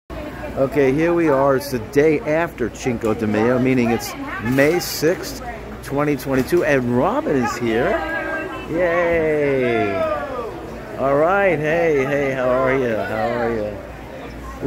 1.) Ocean One Lunch Treat By Billionaire Buddy Brad - May 6th, 2022
Our weekly lunch treat by Billionaire Buddy Brad continues on this T*G*I*F* celebration of friends joyfully bonding to begin our ritual of letting loose and getting a bit wild to express the comic encounters we share in our daily lives over this Happy Hour Lunch with 3 drinks for the price of 1.
Eat up well we did with appetizers, entrees, drinks, and desserts filling our bellies as well as our souls that were coupled with laughter as the best medicine to nurture our hearts & minds. Kudos to Brad for making this weekly wonder a success as usual as we journey down the path of life with humor and uplifting spirits to start the day! 🥂🍾🤸‍♀️🍝🍷🥁🎺🍹😎🍕🥂🍾🤸‍♀️🤪